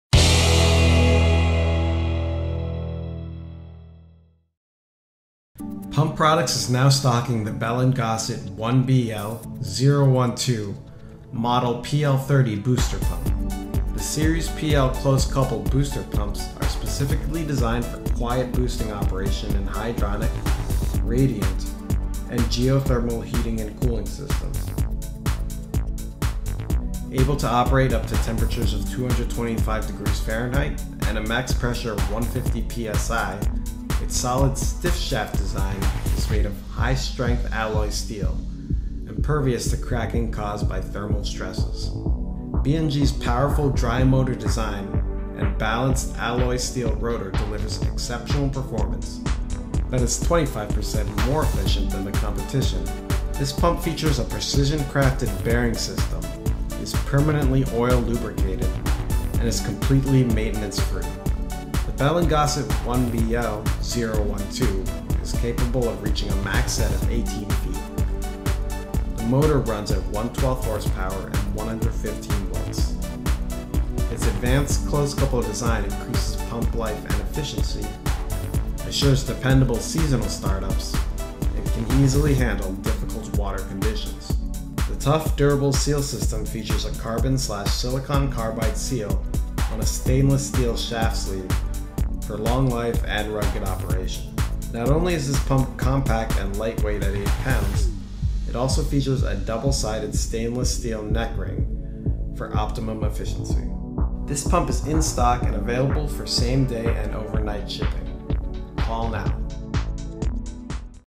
Bell & Gossett 1BL012 Booster Pump Product Highlight
In this video, Pump Products highlights the Bell & Gossett 1BL012 booster pump. This pump is well-suited for pressure boosting and circulation in hydronic heating systems.

Video includes all relevant features, specifications and benefits.

Pump Products' applications engineers are happy to work with customers to figure out the right pump for your application and budget.